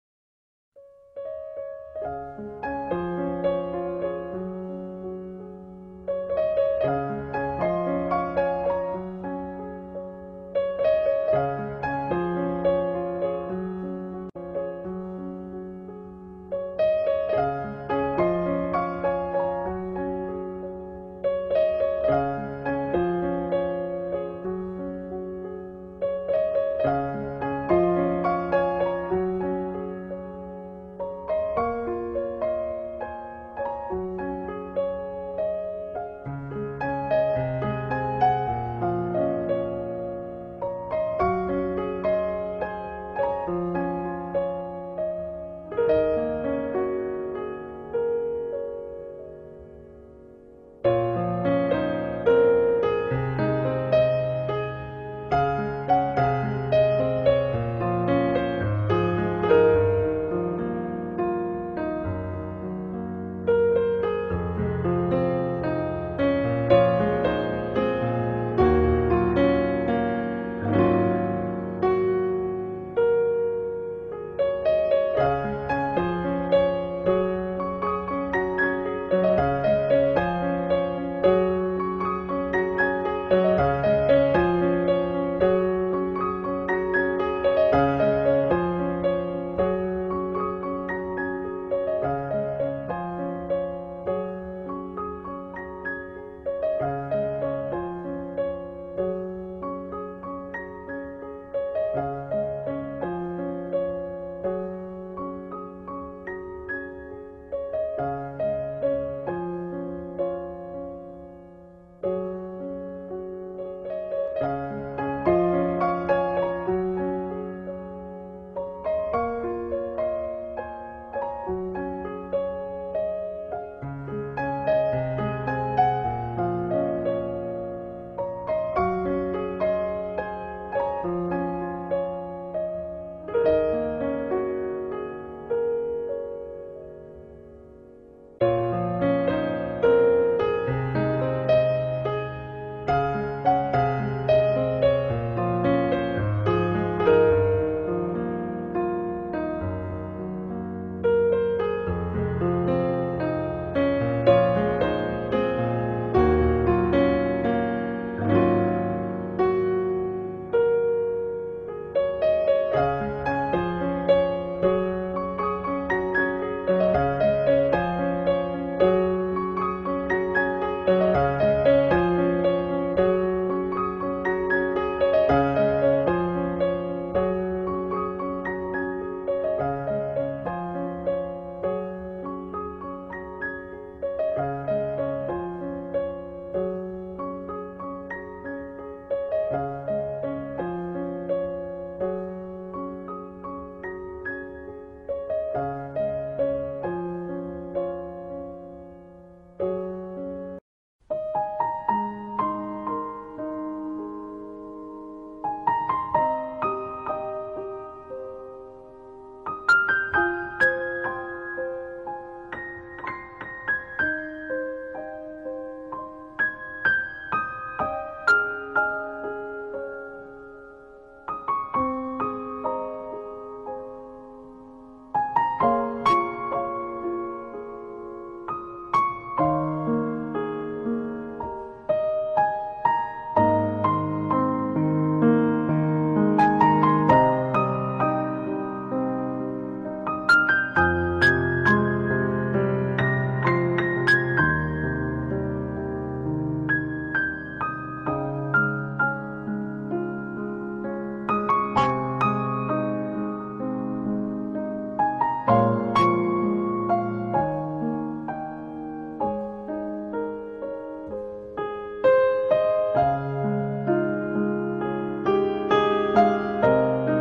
Yiruma Singapore Concert 2014
Blogger Yumiko waited for years for her favourite pianist to come to Singapore and she is way happy to see Yiruma perform Live in front of her on the 5 April 2014!